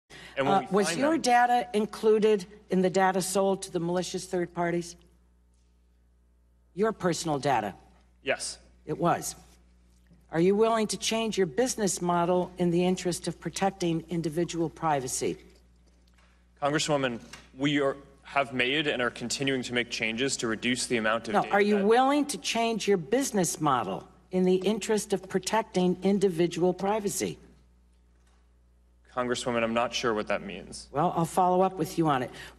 Zuckerberg: My data was included in data sold to Cambridge A
Facebook CEO Mark Zuckerberg says his personal information was included in the data sold to Cambridge Analytica.